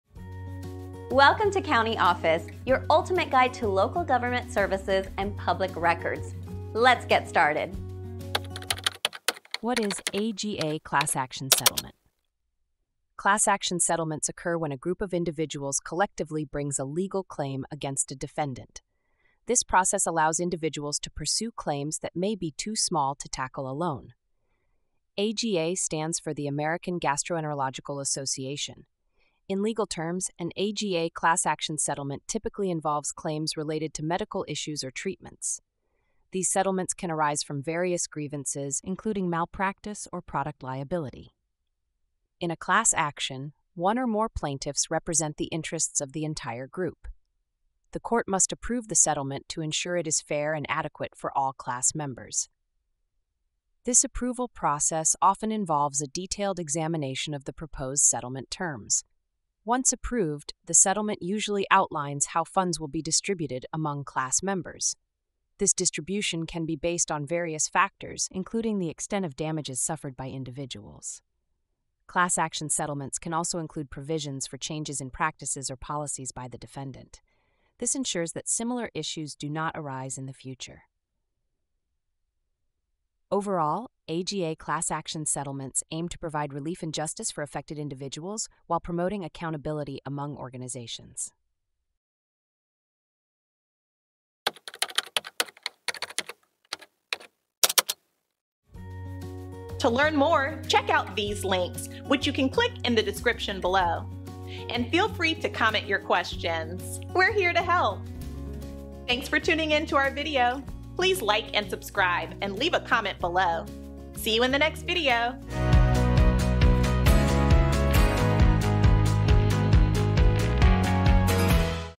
What Is AGA Class Action Settlement? - CountyOffice.org
What Is AGA Class Action Settlement? Class action settlements play a crucial role in the legal landscape, especially when it comes to addressing grievances related to medical issues or treatments. In this enlightening video, we delve into the realm of AGA class action settlements, shedding light on their significance and impact. Discover how these settlements empower individuals to seek justice collectively and hold organizations accountable for their actions.

Learn about the American Gastroenterological Association (AGA) and how class action settlements involving AGA typically unfold. Understand the intricate process of representing a group's interests in court and the meticulous scrutiny settlements undergo for fairness and adequacy. Explore how funds are distributed among class members and the potential for implementing changes in defendant practices to prevent future issues.

Join us as we unravel the complexities of AGA class action settlements, aiming to provide relief and justice for affected individuals while fostering accountability within organizations.

He